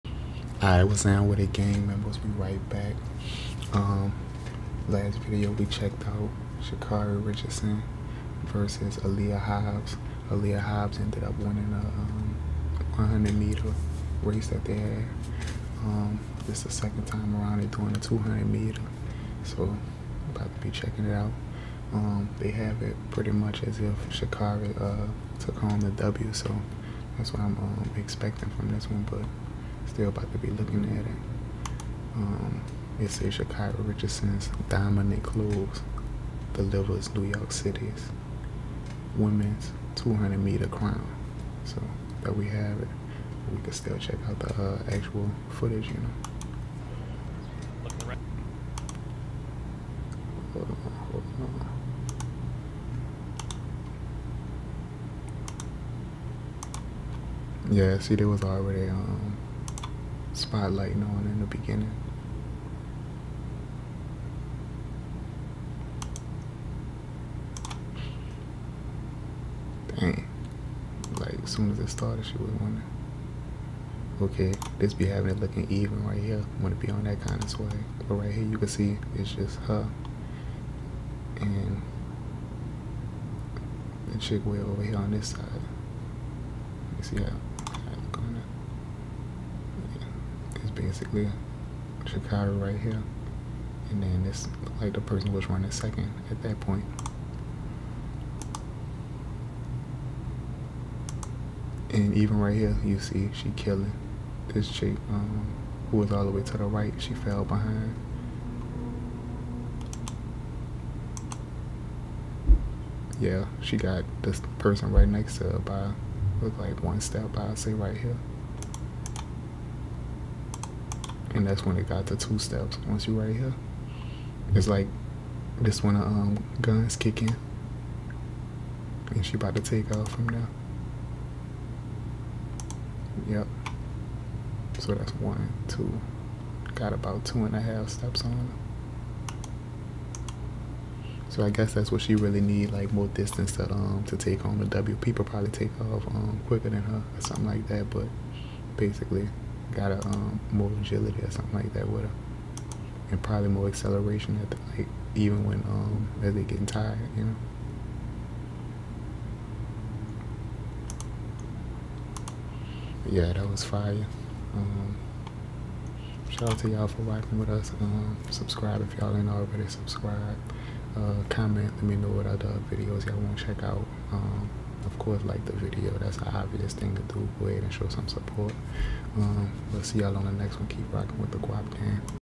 Sha’Carri Richardson's DOMINANT close delivers NYC women's 200m crown | NBC Sports
Be The First To Comment Sha’Carri Richardson's DOMINANT close delivers NYC women's 200m crown | NBC Sports